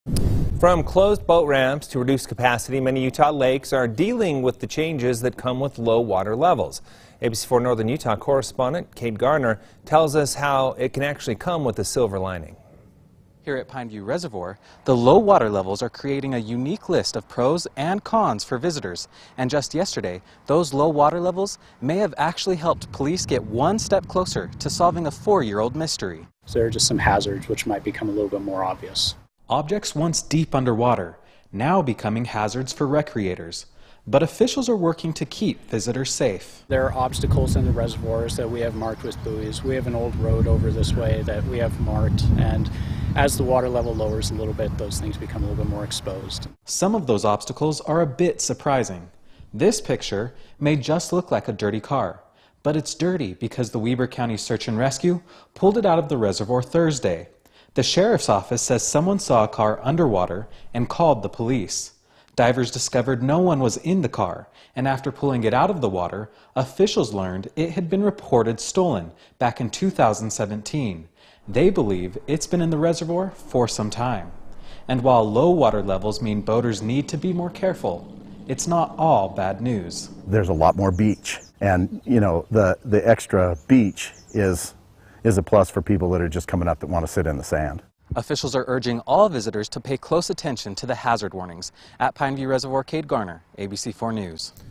Water Levels Reveal Hidden Dangers
The drought has caused low water levels at Pineview Reservoir. This is changing the way people can recreate as well as revealing hazards in the water.